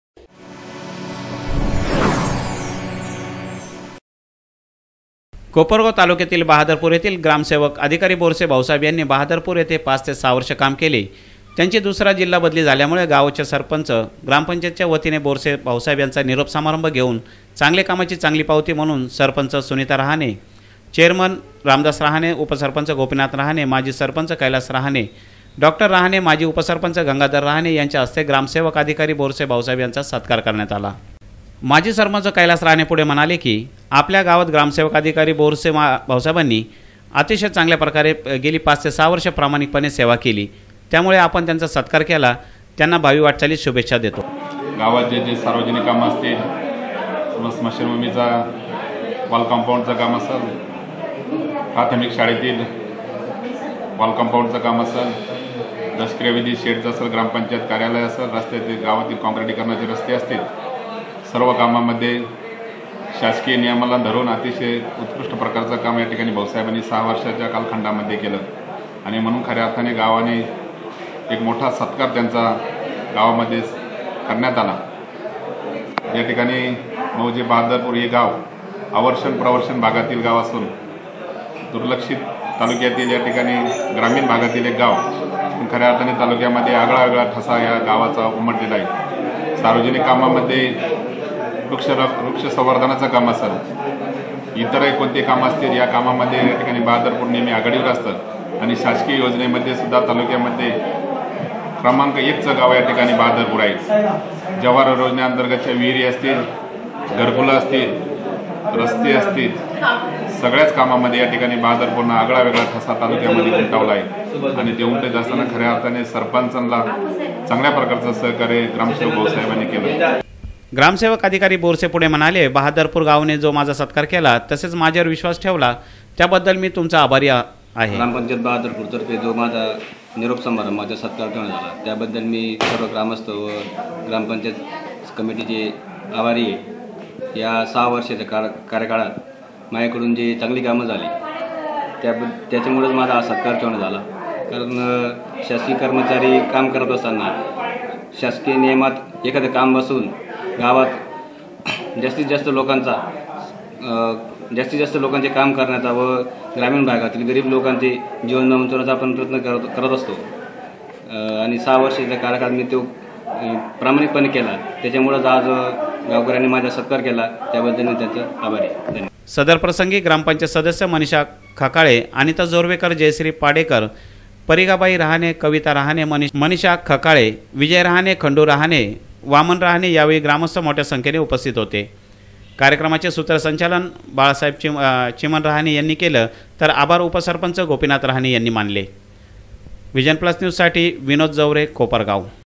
कोपरगाव- बहादरपुर ग्रामसेवक भाऊसाहेब बोरसे यांचा बदलीनिमित्त सत्कार
कोपरगाव- बहादरपुर ग्रामसेवक भाऊसाहेब बोरसे यांचा बदलीनिमित्त सत्कार करण्यात आला..बोरसे यांनी या ठिकाणी चार ते पाच वर्षे प्रामाणिकपणे सेवा केली.

सर्वसमावेशक आढावा घेणारी जिल्हास्तरीय निर्भीड वृत्तवाहिनी..व्हिजन प्लस न्यूज..